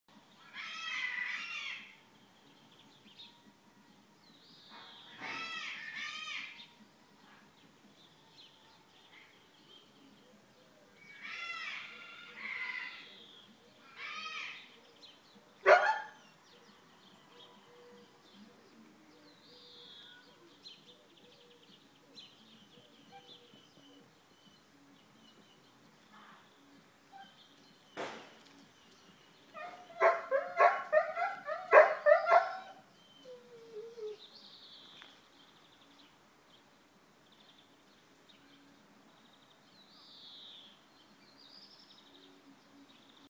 Noortje in de boom.
Het liefst vliegt ze erin en weer ui!